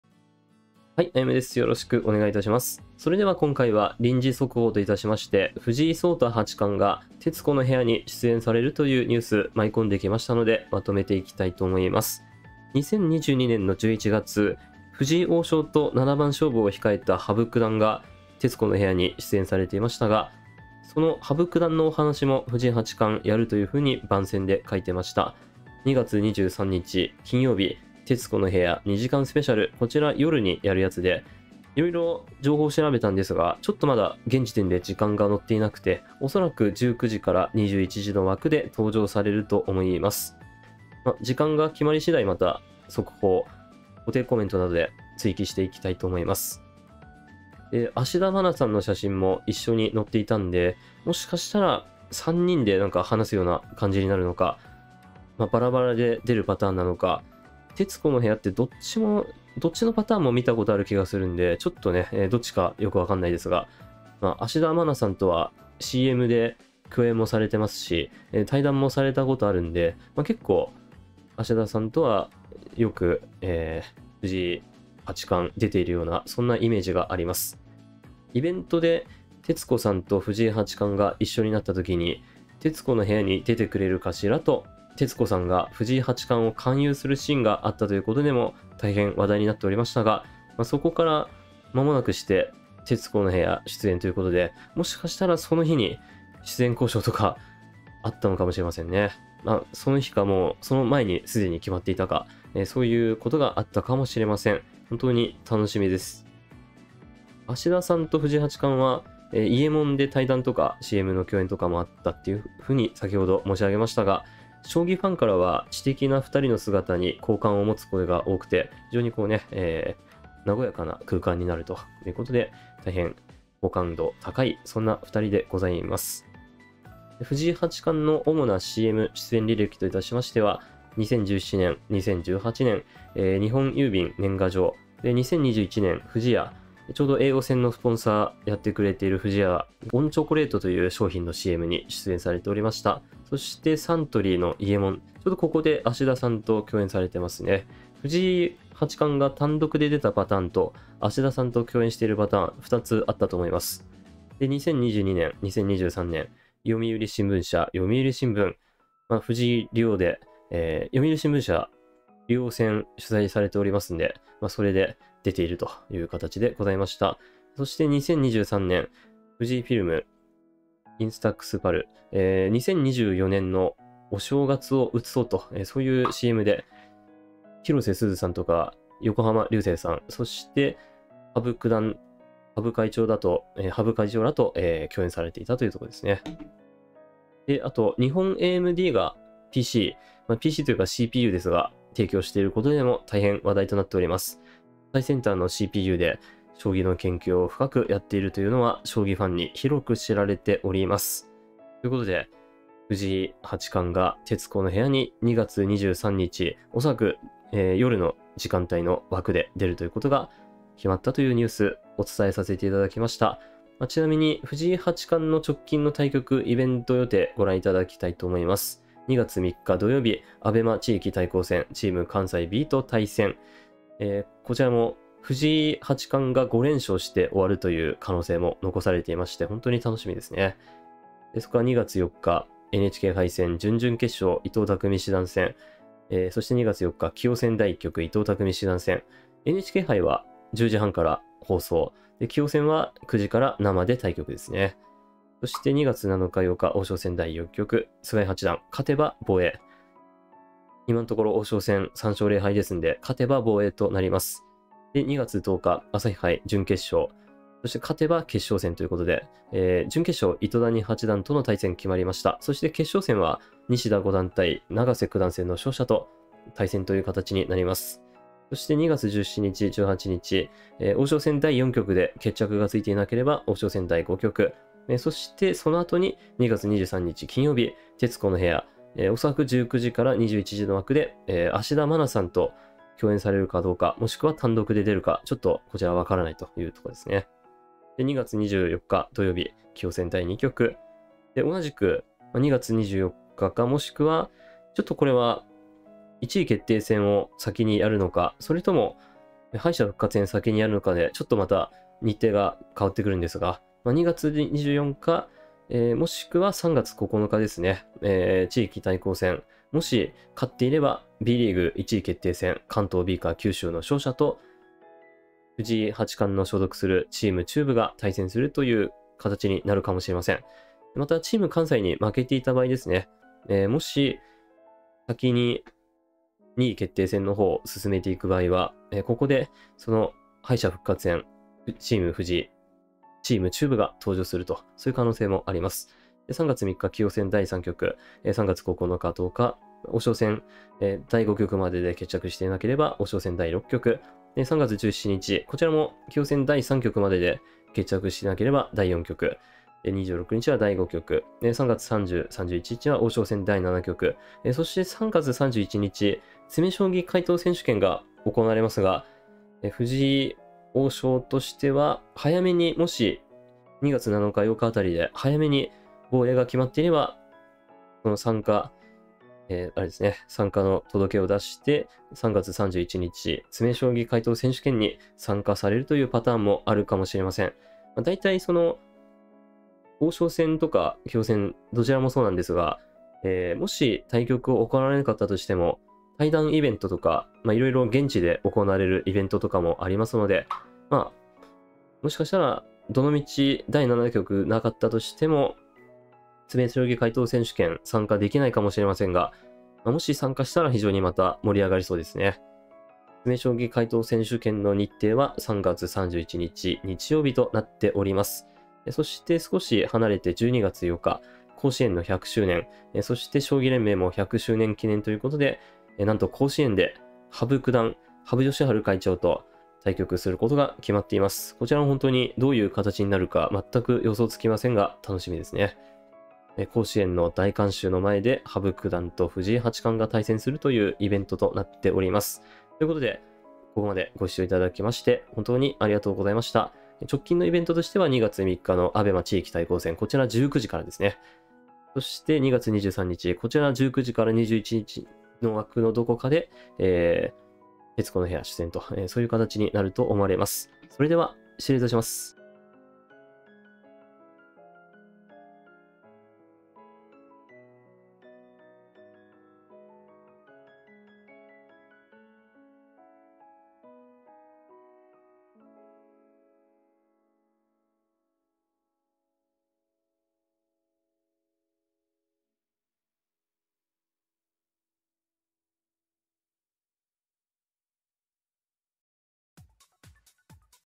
【臨時速報】藤井聡太八冠、徹子の部屋に出演決定！幼少期、普段、羽生善治九段の話など 芦田愛菜さんと共演？
2月23日の夜の枠でテレビ朝日にて放送されます。